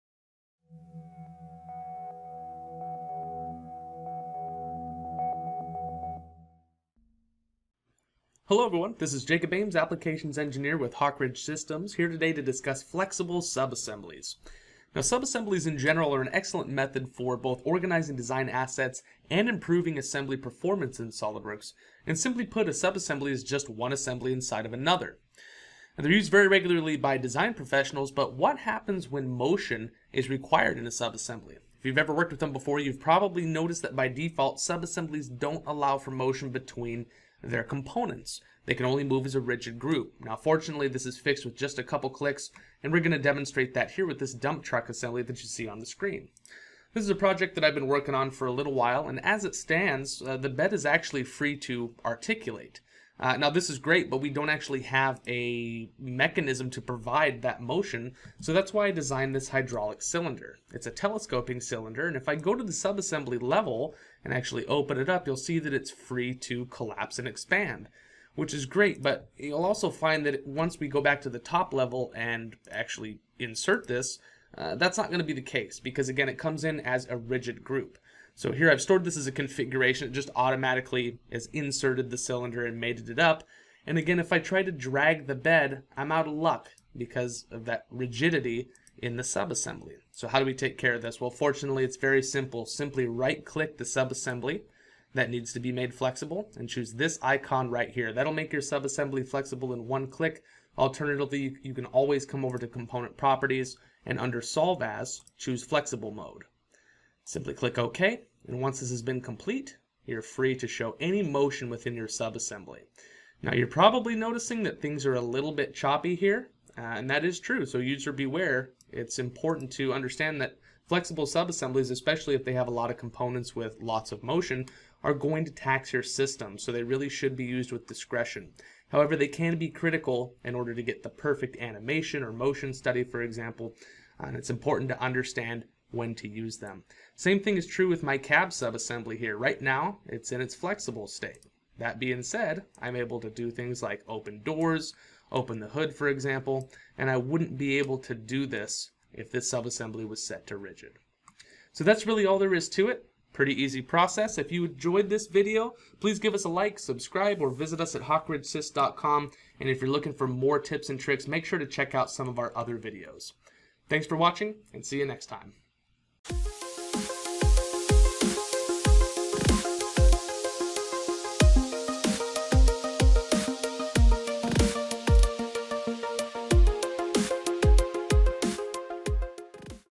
SOLIDWORKS: Making Subassemblies Flexible
Ever run into a subassembly that just won't move the way you want? Here, we show you a simple fix to make your subassemblies flexible, allowing them to move just as you'd expect.